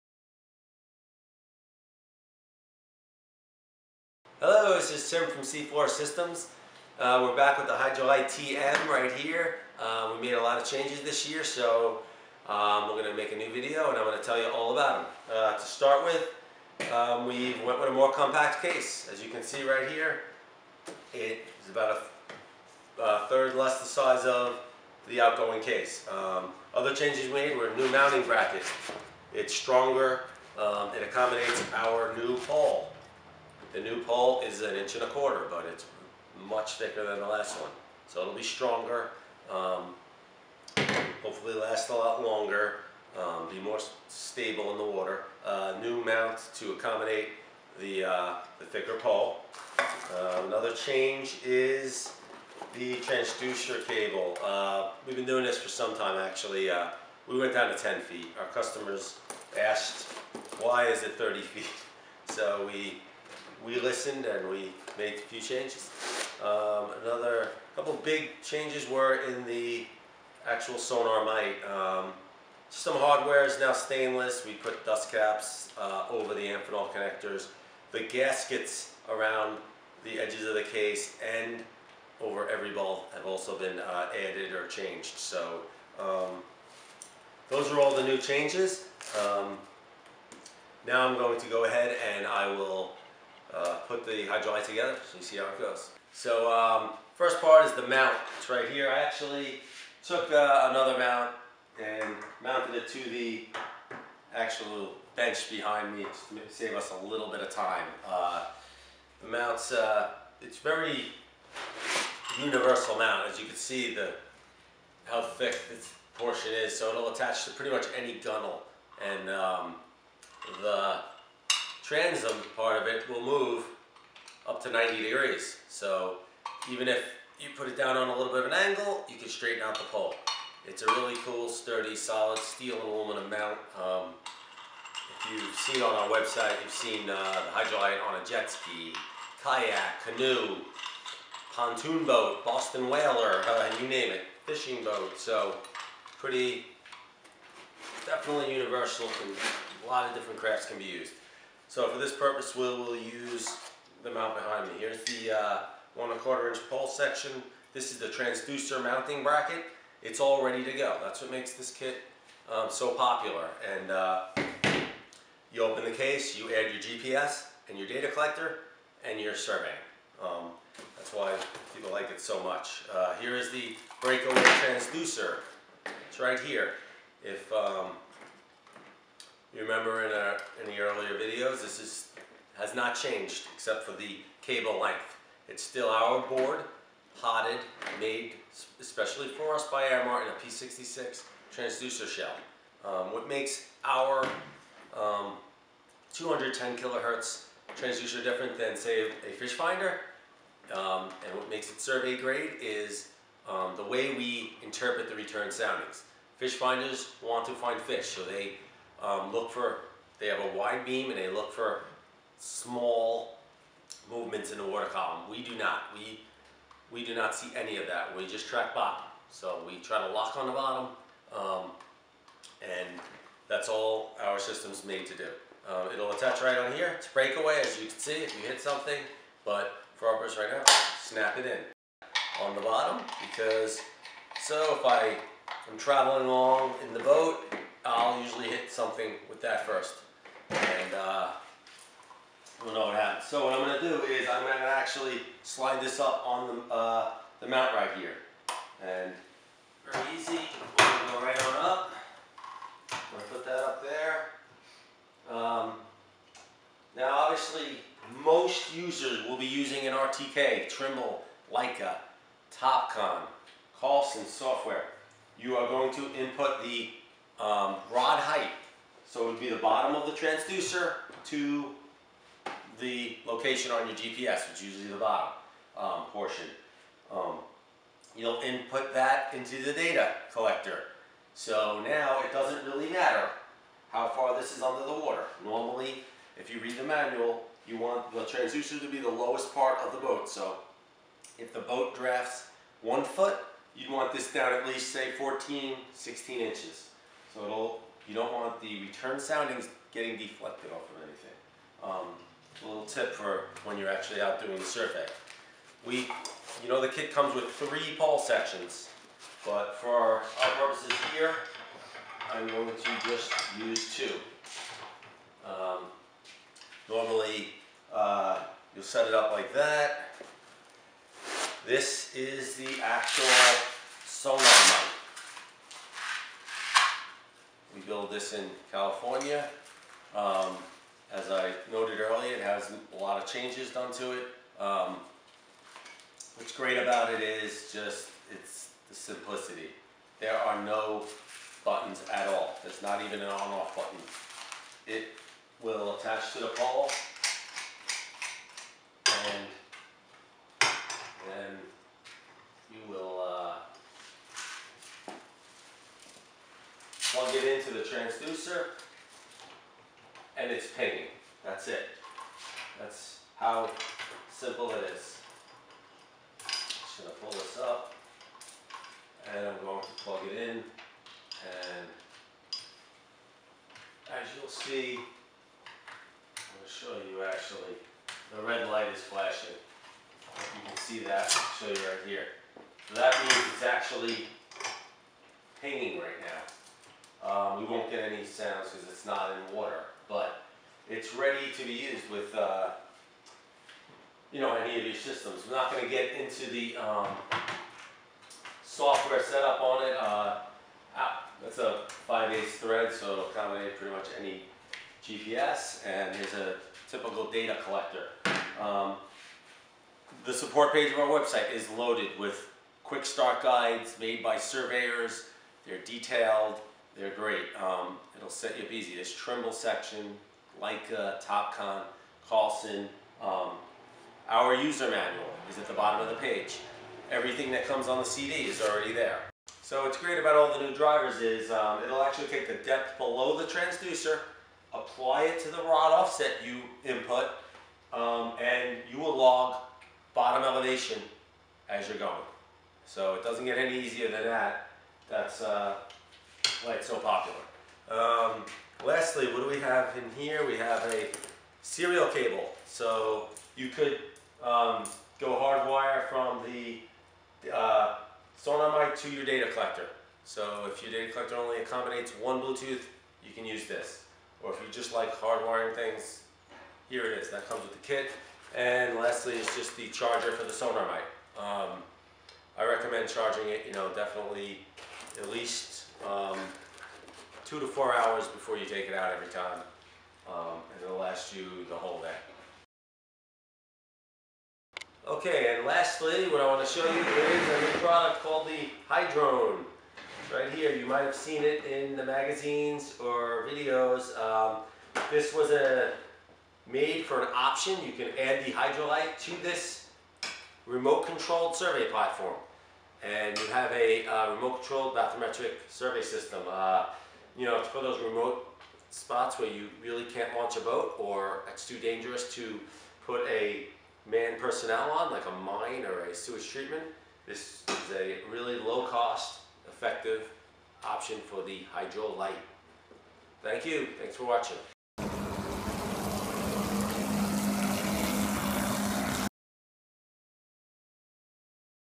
HydroLite-TM Portable Survey System
The HydroLite-TM is a portable, bathymetric survey system. It has been designed as a turnkey, shallow water singlebeam rig. Tim Tamplin demonstrates how to set up the system.